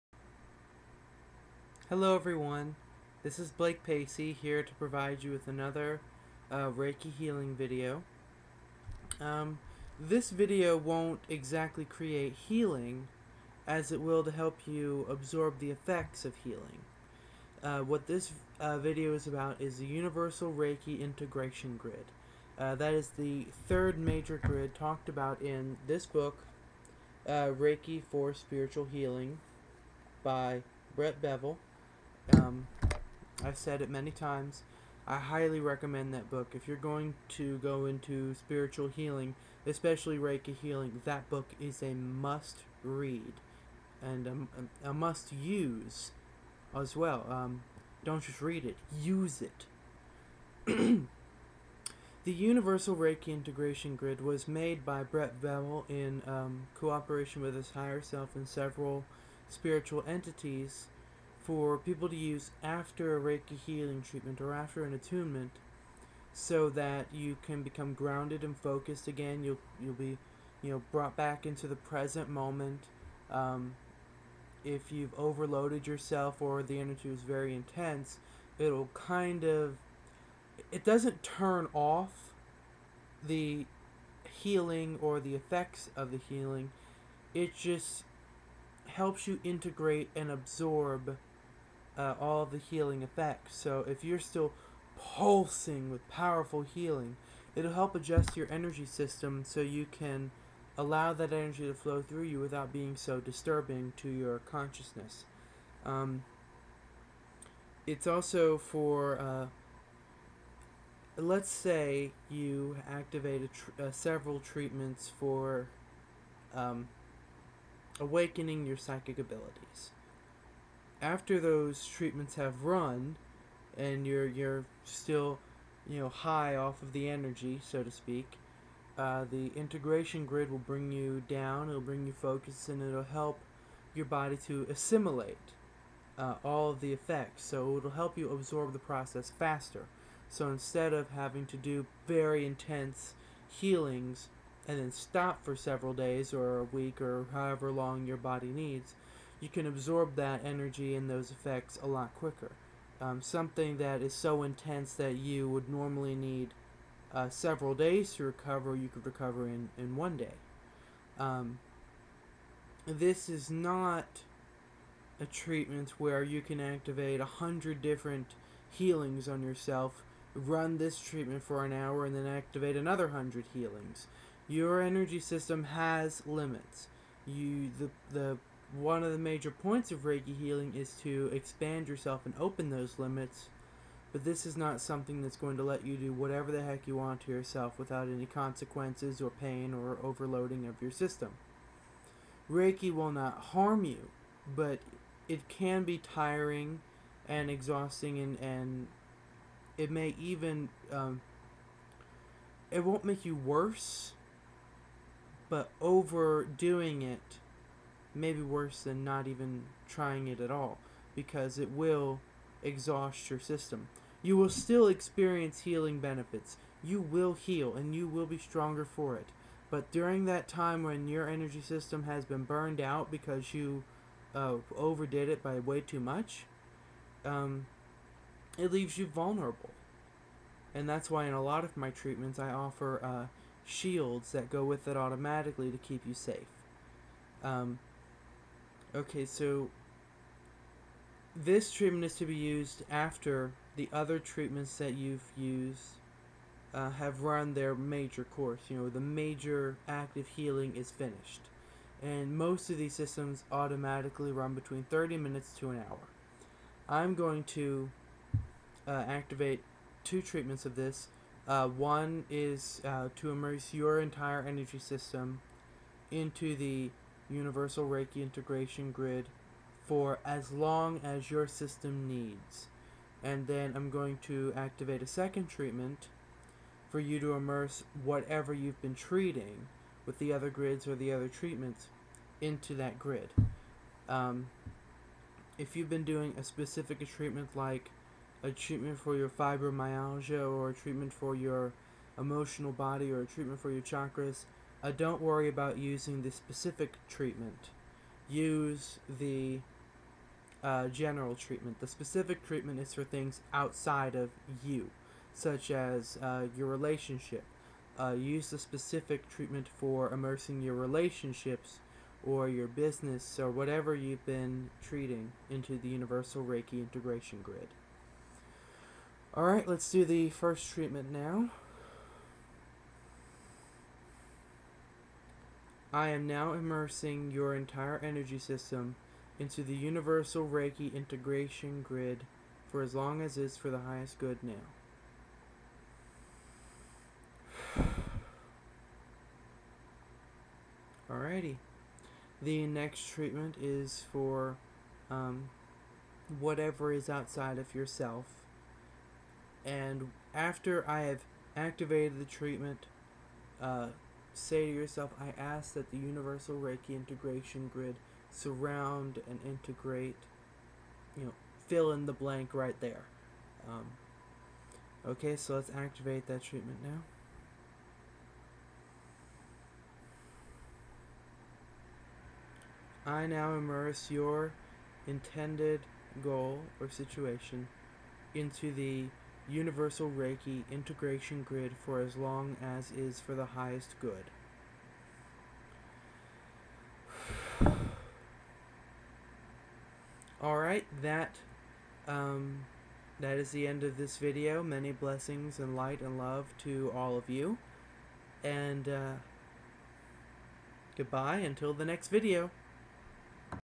Universal Reiki Integration Grid
This video explains and uses the Universal Reiki Integration Grid for assimilating and absorbing the effects of healings & attunements quicker, without overloading your system. Do not abuse your energy system just because you have access to this treatment. This treatment also helps with grounding and coming into the present moment. Great after meditating or performing a ritual or spell and you need to get back down to earth. Love to you all!